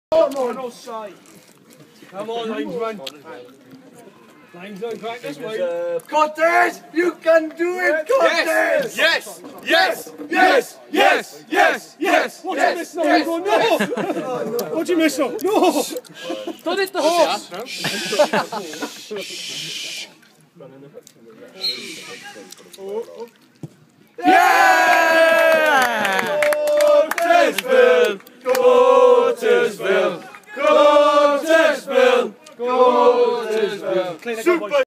Cortez Belle Penalty v Briton Ferry
August 4th- Pre-season friendly.
Briton Ferry 1-5 Port Talbot
Cortez Belle penalty, 1st of his hat trick, 3rd of our five. Away days with Port Talbot Town FC. 1901 Ultras.